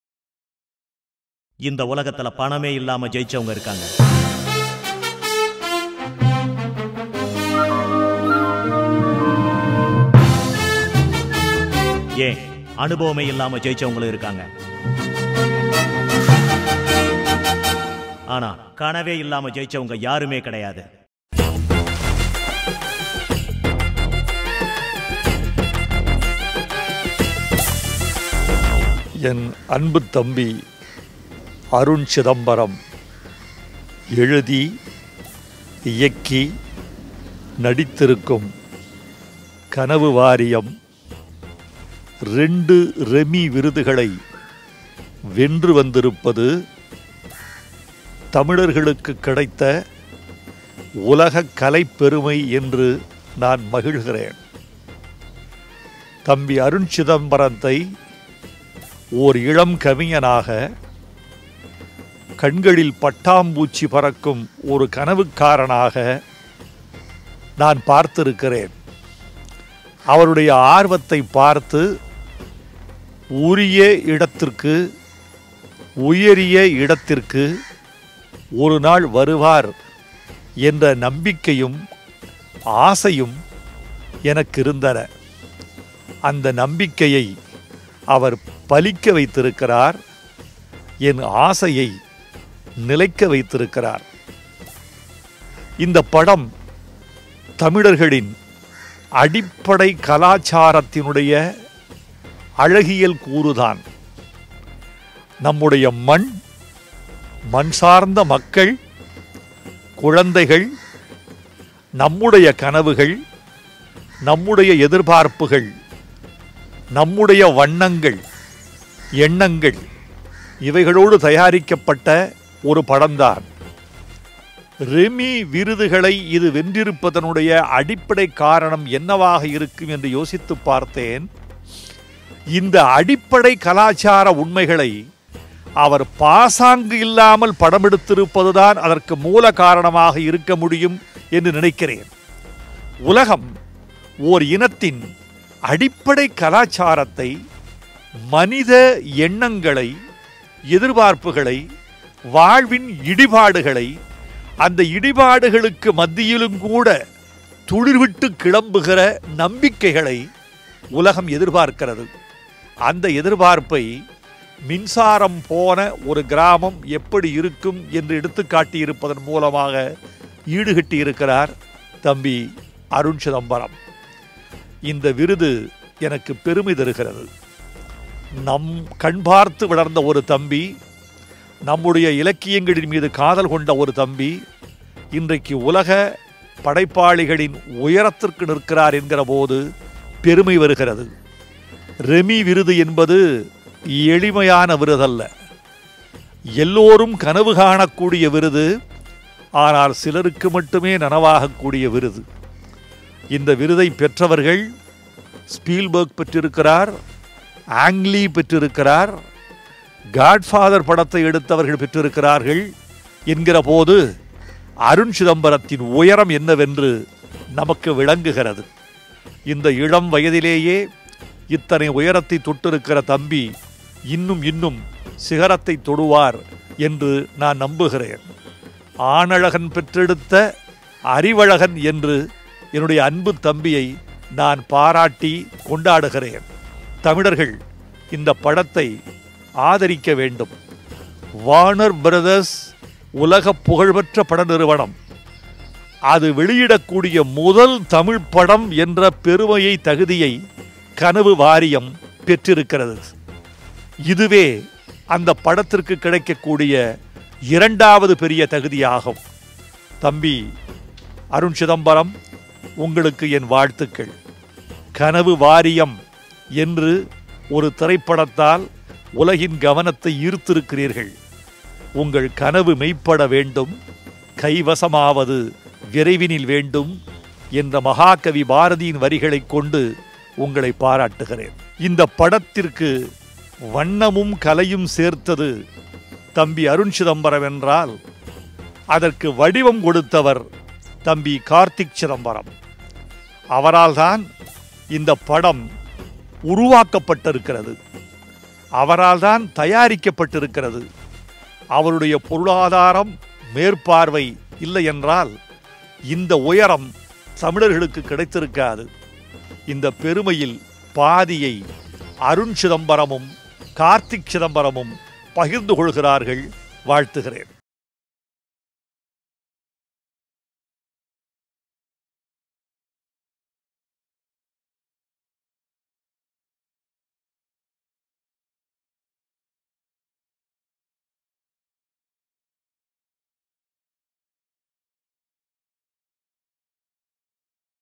Kanavu Variyam makes Tamilans feel proud : Vairamuthu | Warner Bros Release
Most Dominant Tamil Kavignar of Tamizh's Soul Kavignar Vairamuthu is here with us to Share his Feel about Kanavu Variyam.
Kanavu Variyam IHaveADream, An inspiring entertainer based on power-cuts in Tamil Nadu. This is the first Tamil and South Indian film to be distributed by Warner Bros. Pictures. The movie has won 7 International awards and 15 Int'l Honours from 9 Countries. Kanavu Variyam is the first Indian Film to win 2 Remi Awards.

Previous recipients of Remi award include Steven Spielberg (Jurassic Park), Ang Lee (Life of Pi), Ridley Scott (The Gladiator), George Lucas (Star Wars), Francis Ford Coppola (The GodFather). Arun Chidambaram is the first Indian Director to win 2 Remi Awards.

Star Cast: Arun Chidambaram, Jiya, Ilavarasu, Black Pandy, Prof.Ku.Gnanasambandan, Yog Japee and Sendhi Kumari.
Story, Screenplay, Dialogues, Lyrics & Direction: Arun Chidambaram
Produced By: Dr. A. Chidambaram, Karthik Chidambaram | DCKAP Cinemas
Cinematography: S.Selvakumar
Editing: Gaugin.

KanavuVariyam Teaser was launched by Yuvan Shankar Raja.